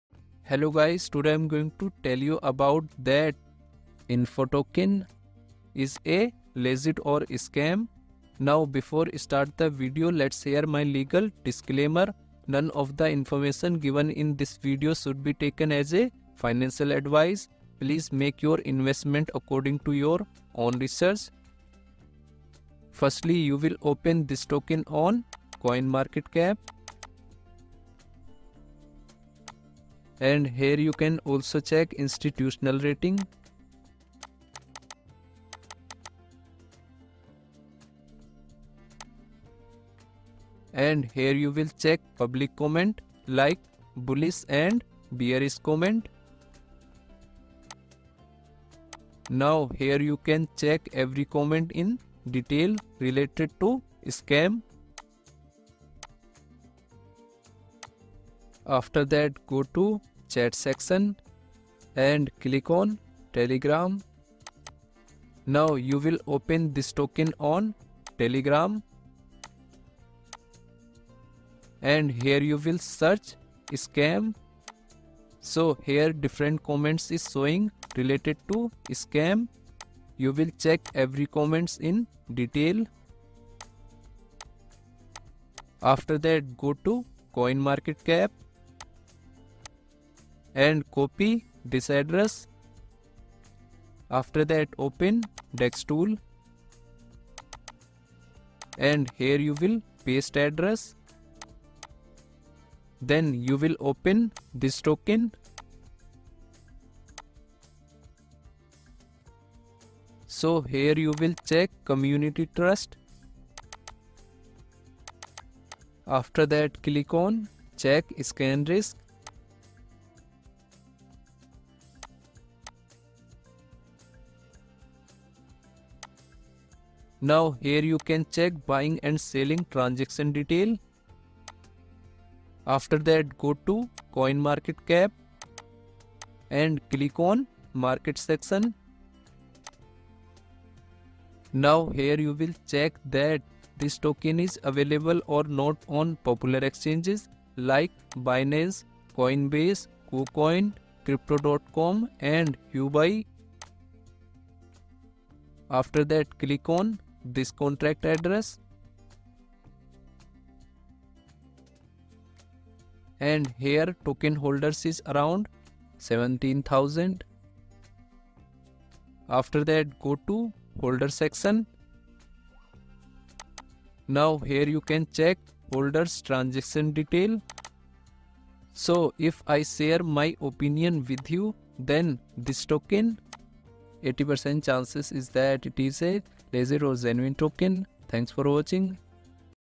Is INOFI (FON) Token Legit or Scam ??
In this video, I am going to share that INOFI (FON) Token is legit or a scam. I have made videos based on online data, I am not a financial advisor investing in crypto at my own risk.